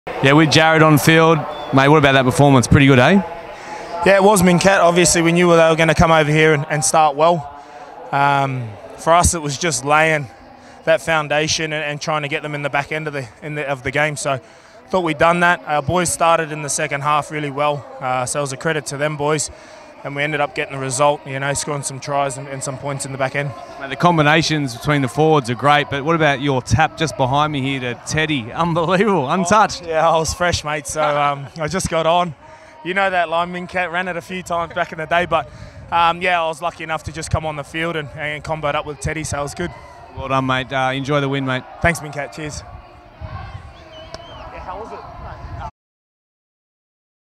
RoostersTV | Post match with JWH
RoostersTV spoke with Jared Waerea-Hargreaves following a strong performance by the Roosters at the SCG in Round 22 against the New Zealand Warriors.